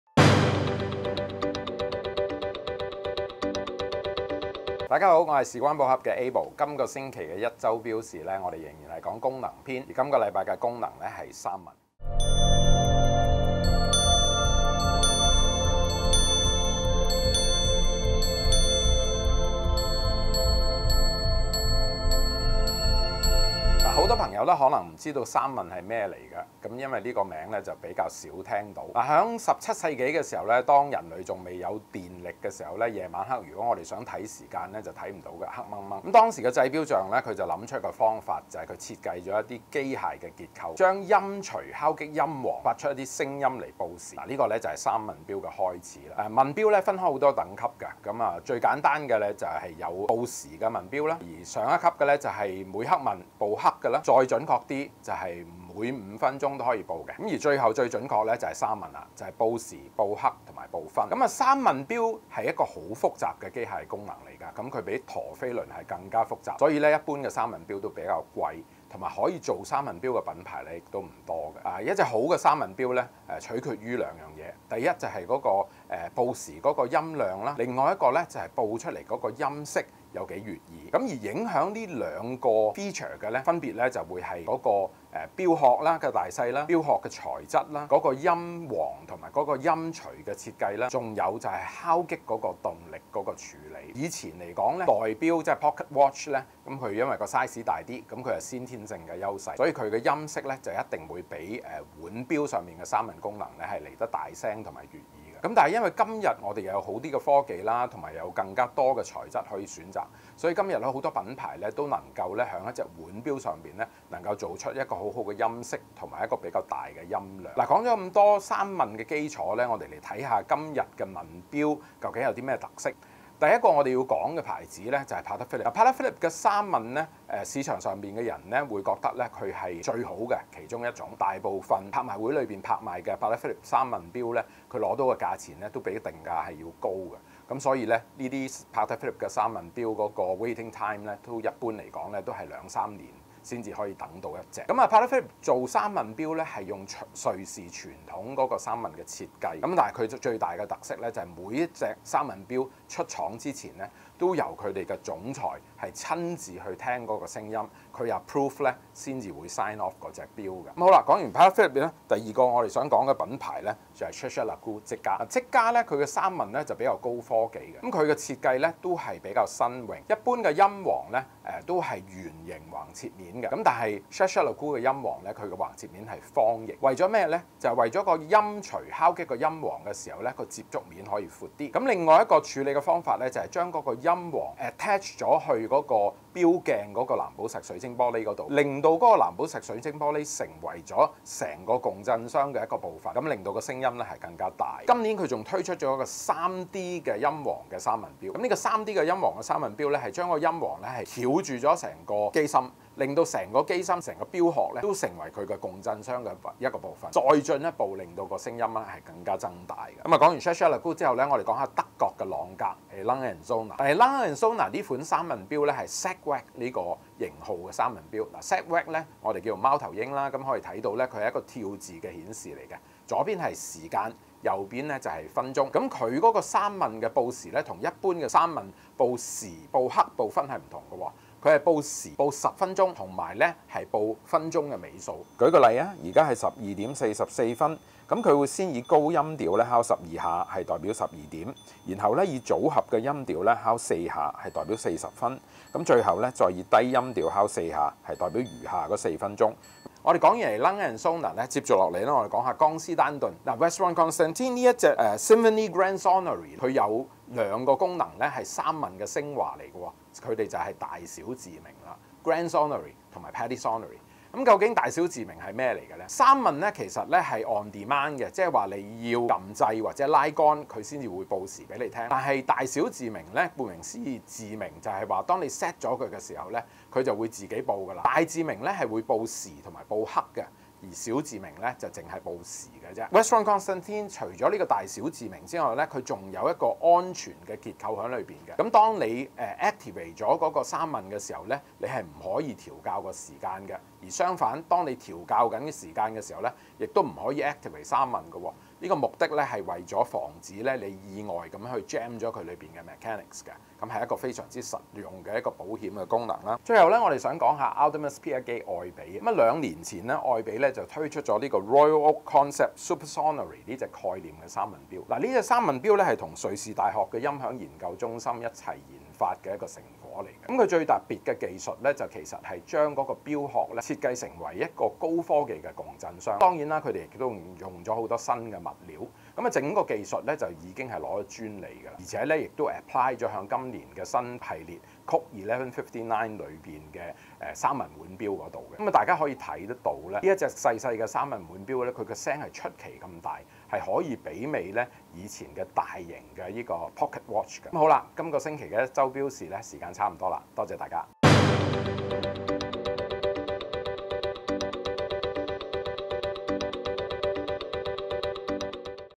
【一周表事回顧】功能篇 三問表
這表值幾百萬，就因為它懂發聲？！
明明，它的出現與你無干，
明明，你根本不需要它，
偏偏，你就要花幾百萬來買一隻表，
開聲聽聽這影片，然後，
準備提款去吧……

bmgproductionmusic